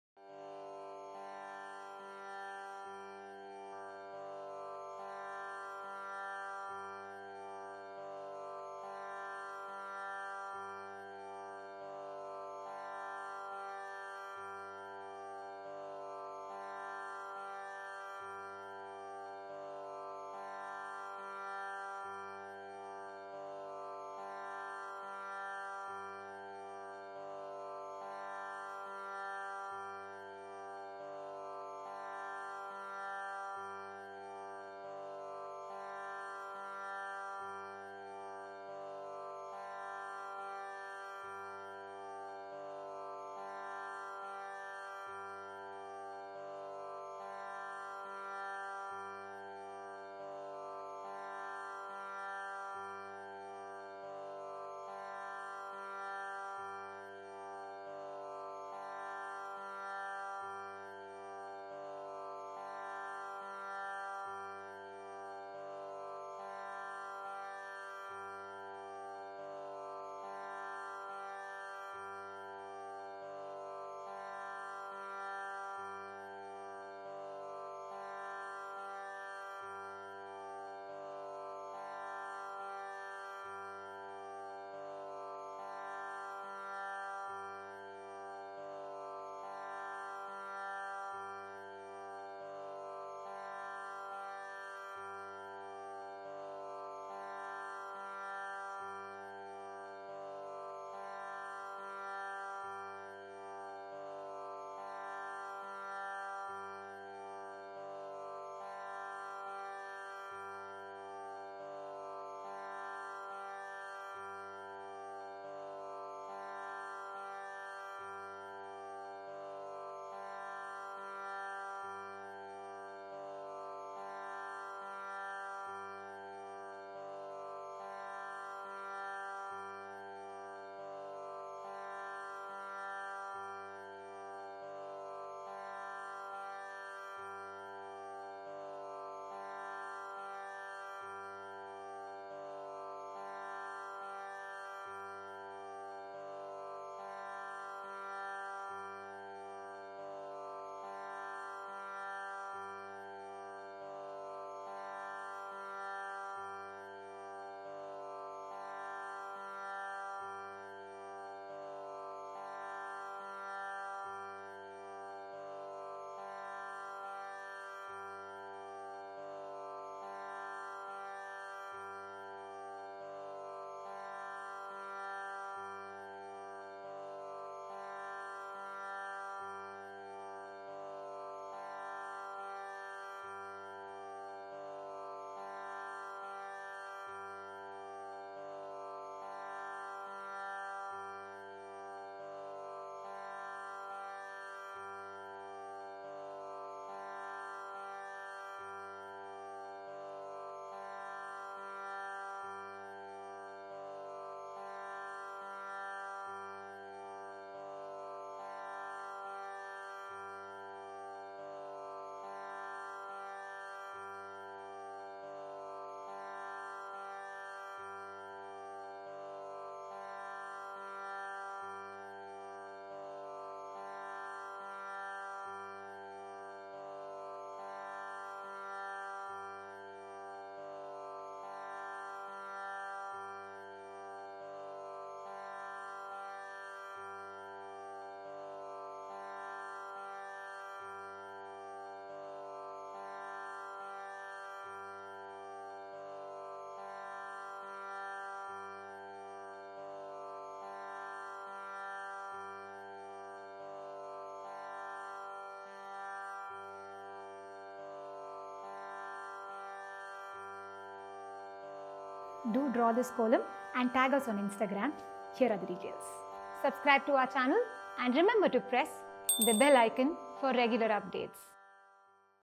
APARTMENT & DOORWAY SIKKU KOLAMS -050 || 2X2 SERIES
Now, here is a collection of Kolams perfect for your doorways and apartment corridors. Who ever said space is a constraint ;)

This collection has been designed from various Bheeja Kolams, for you to be able to appreciate the design.